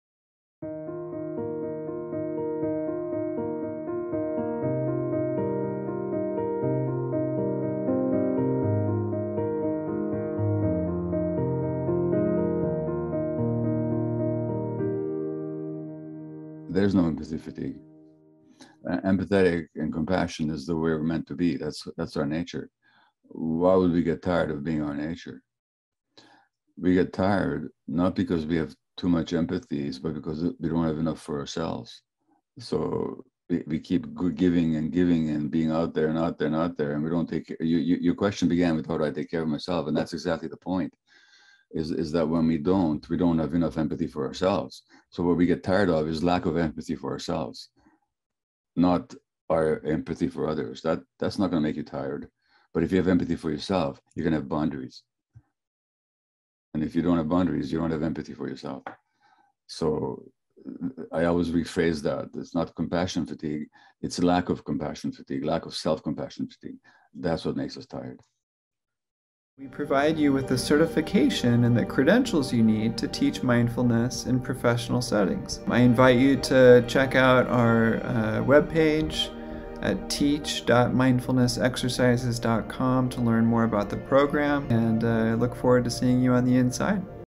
There Is No Compassion Fatigue – Dr Gabor Maté
Therapists and caregivers often speak of compassion fatigue or empathy fatigue. Dr. Gabor Maté explains why there is no such fatigue – and that rather, there is another reason we get tired when supporting others.

This clip comes from Dr. Gabor Maté’s second workshop as part of the Mindfulness Meditation Teacher Training Program. Learn more about how you can certify to teach mindfulness meditation: